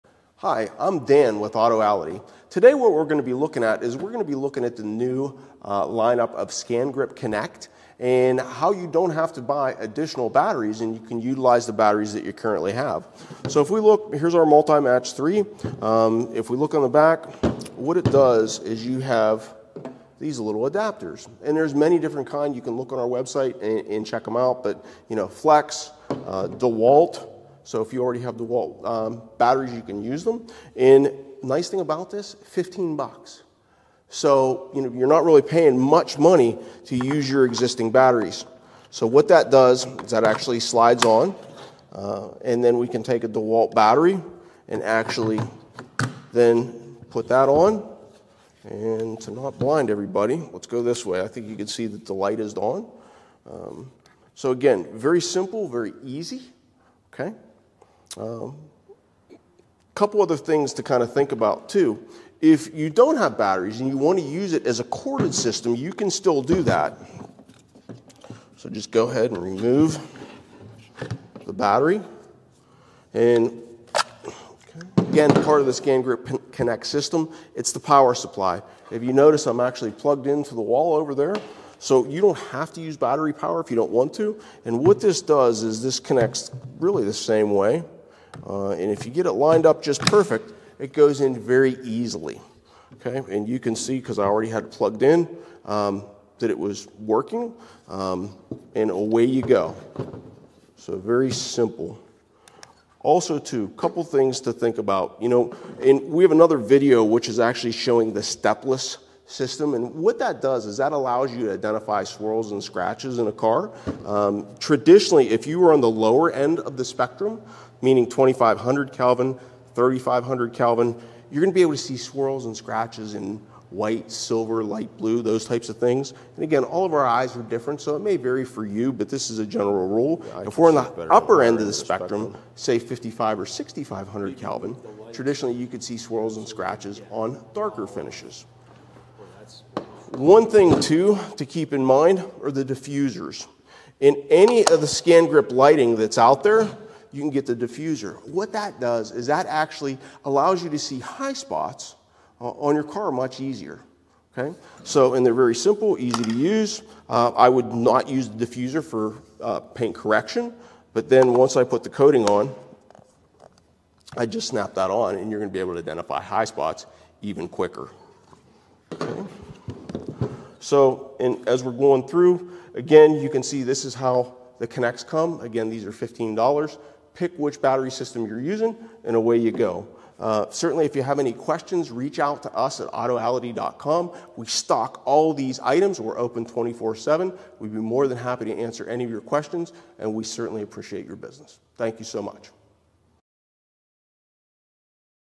ScanGrip Connect at Autoality
ScanGrip Connect lets you use your own tool batteries with easy to use adapters! Dan shows us how simple it is to use